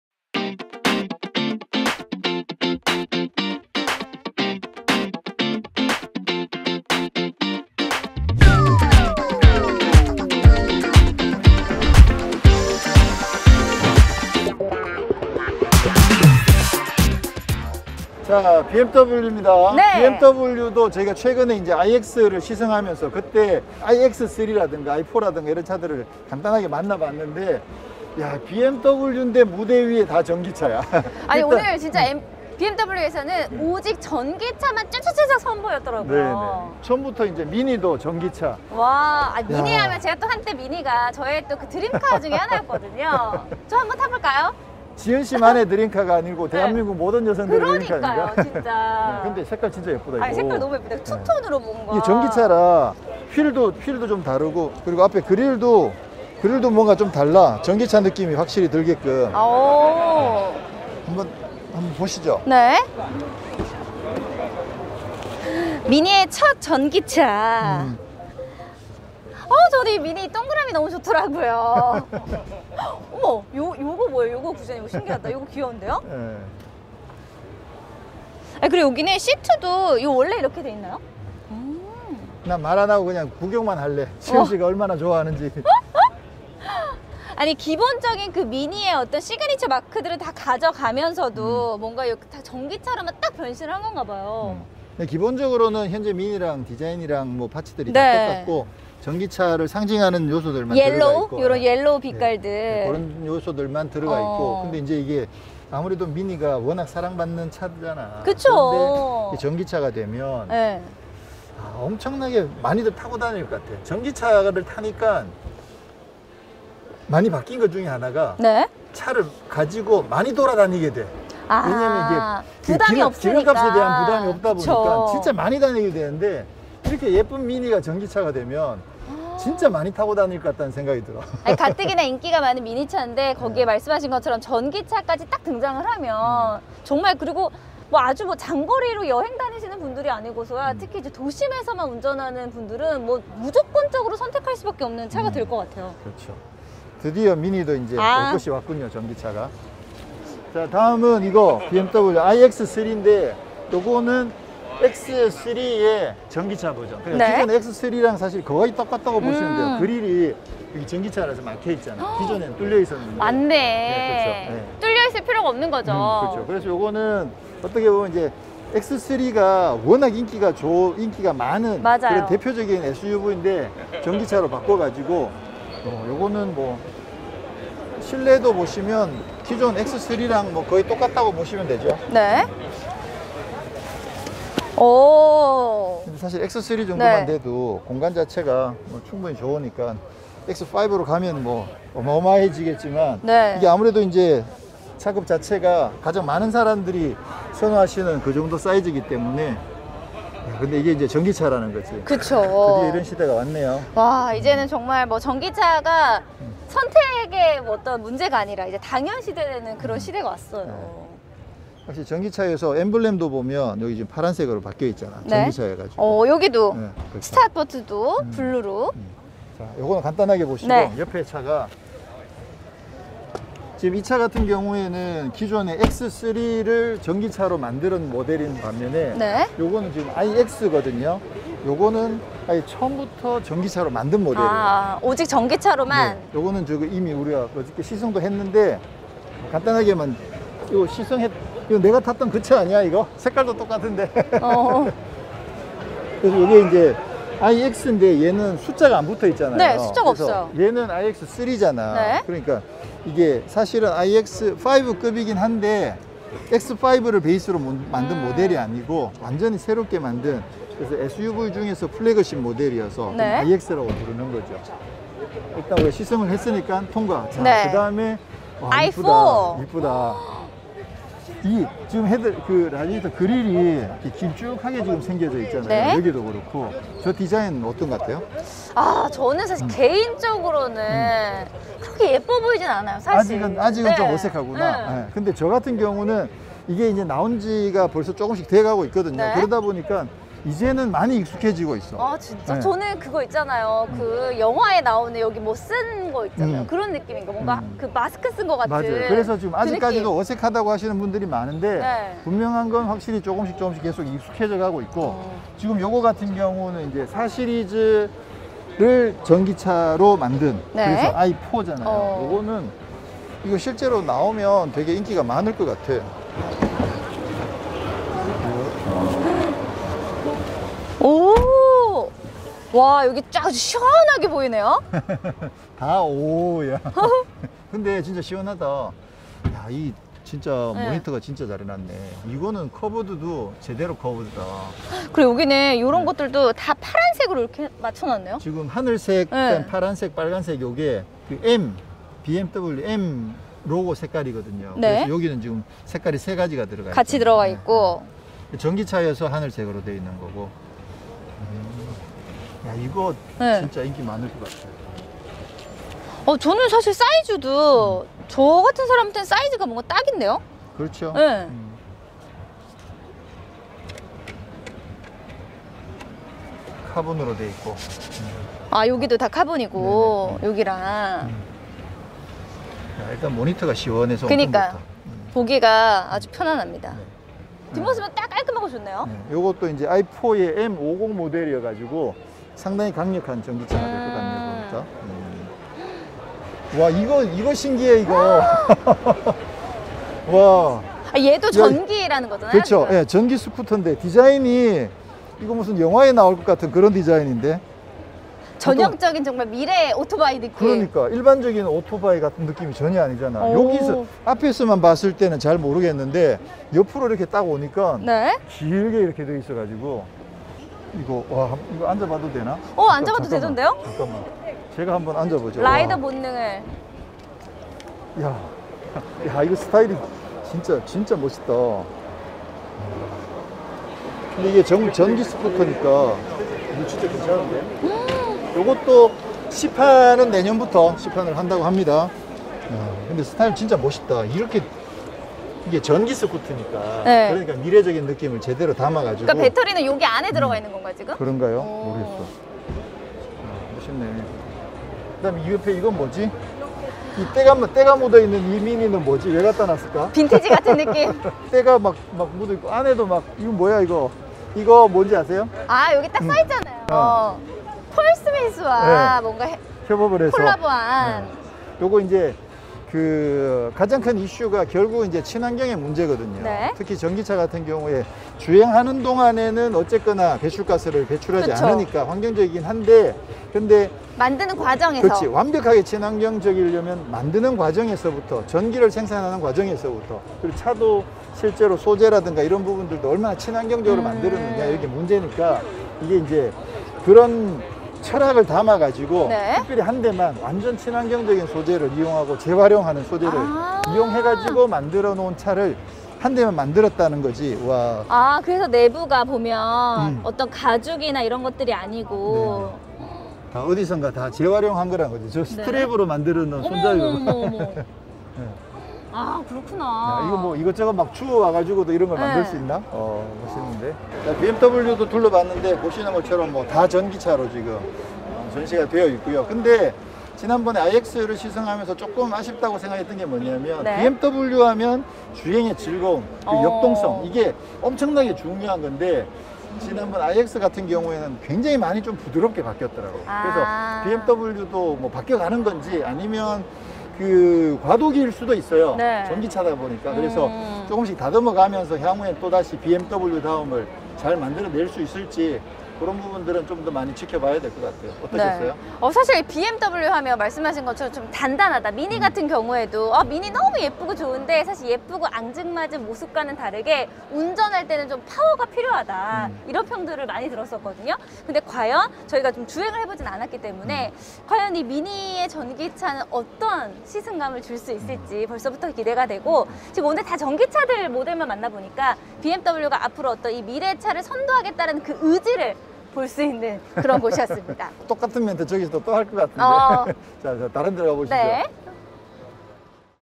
미니도 스쿠터도 전기차! BMW iX, iX3, i4 2021 서울모빌리티쇼 BMW관
2021 서울모빌리티쇼 BMW관에도 온통 전기차가 전시됐습니다.
최근 시승한 iX를 비롯해, iX3, i4, 미니 일렉트릭, 그리고 전기 스쿠터 CE 04까지.